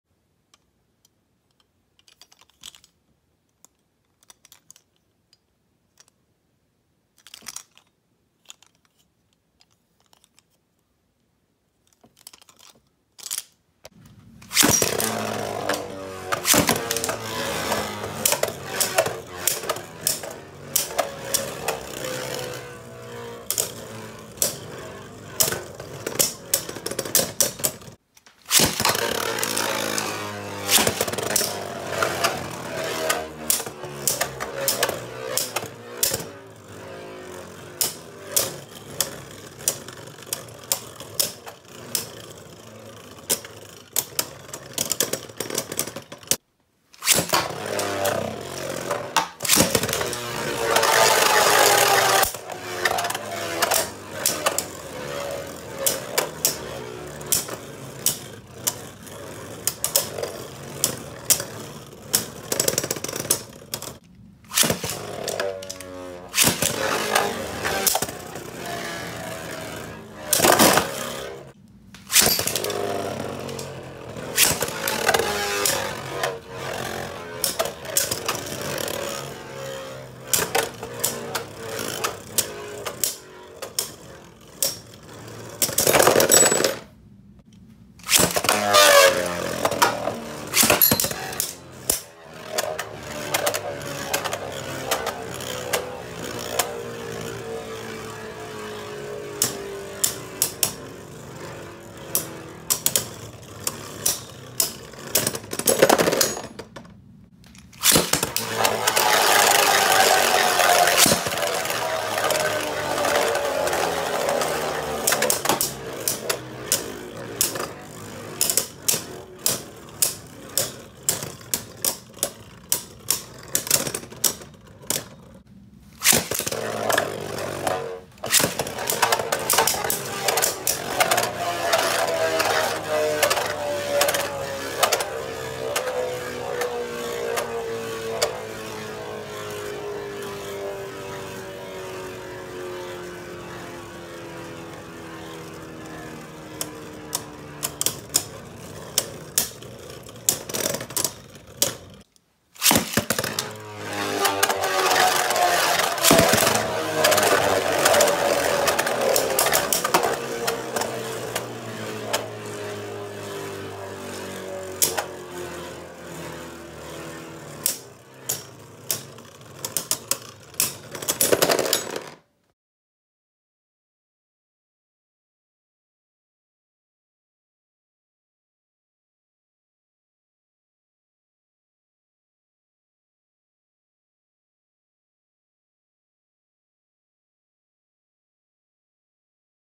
Victory Valkyrie vs Dark Deathscyther | Beyblade Burst
Dark Deathscyther vs Victory Valkyrie  - Dual layers

Full combos: Victory Valkyrie & Dark Deathscyther Force Jaggy

Request a battle in the comments
battles in the Takara Tomy B-09
sakurazeus / sakura zeus beyblade

victory valkyrie vs dark deathscyther
dark deathscyther vs victory valkyrie This video is made for a audience of 13+ .